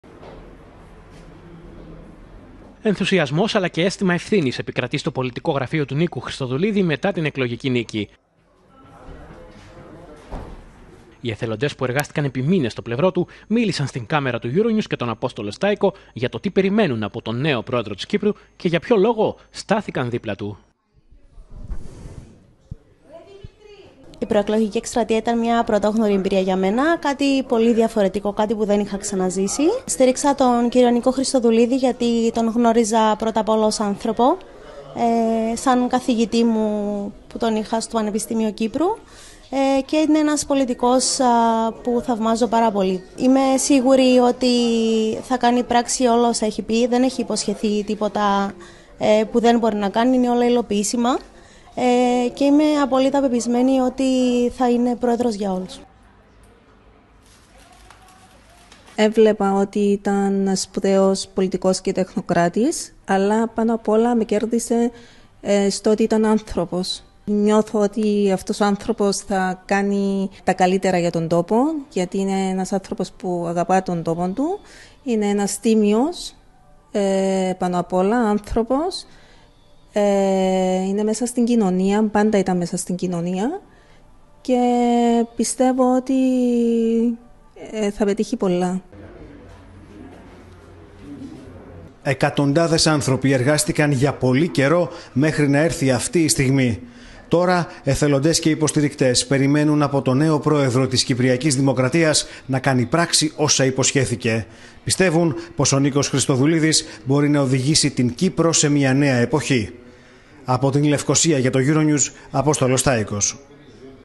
Κύπρος: Οι εθελοντές του Νίκου Χριστοδουλίδη μιλούν στο euronews για τον νέο Πρόεδρο
Γιατί τον πίστεψαν και στάθηκαν στο πλευρό του όλους αυτούς τους μήνες